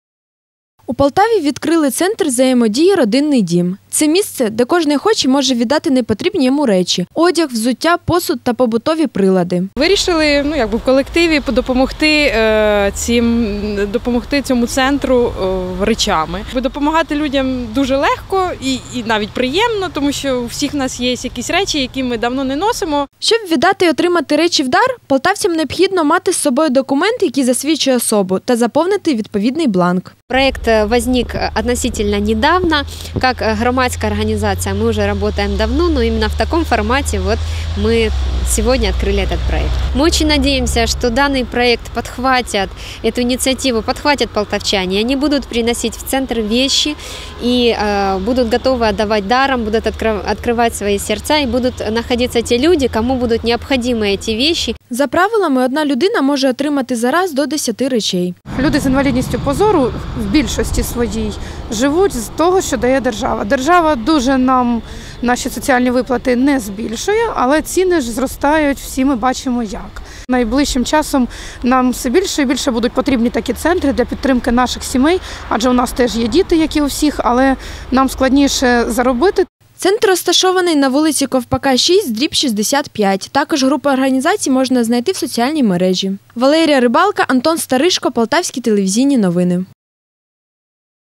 Куди можна віднести речі, що лежать без потреби?
«Віддам даром». Куди можна віднести речі, що лежать без потреби  на полиці. І де можуть отримати речову і моральну допомогу ті, хто цього потребує, – розкажуть наші журналісти.